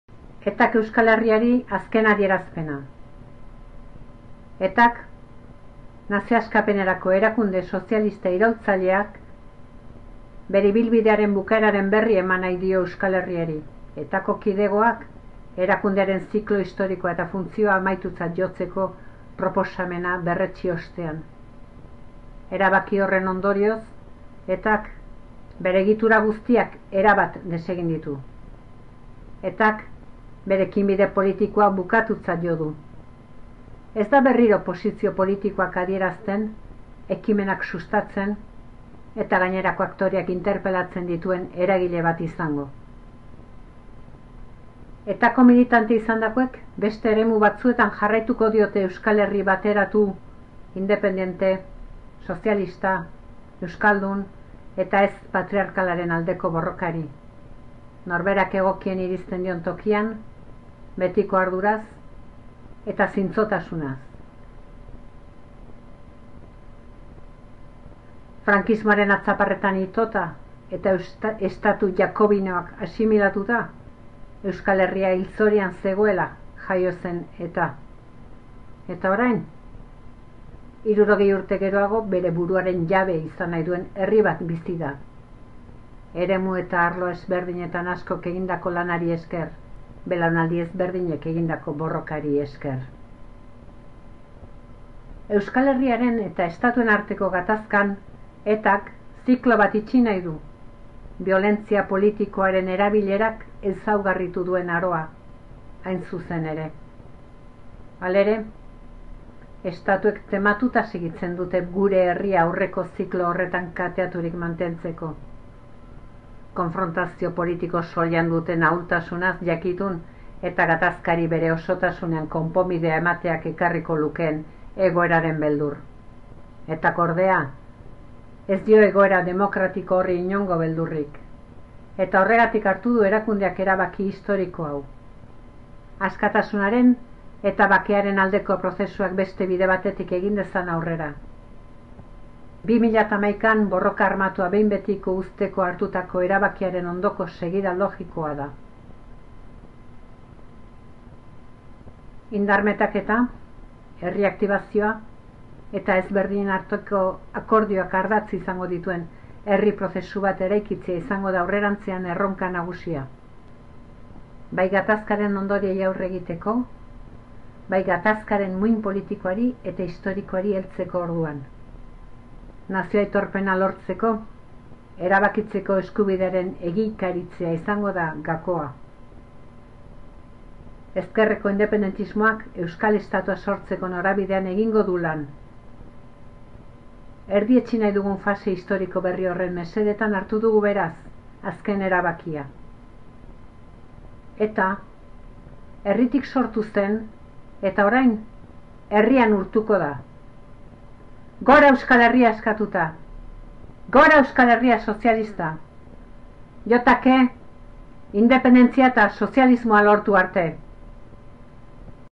ETAren azken agiria
ETAren azken adierazpena, Marixol Iparragirre euskal preso eta ETAko militante historikoaren ahotan.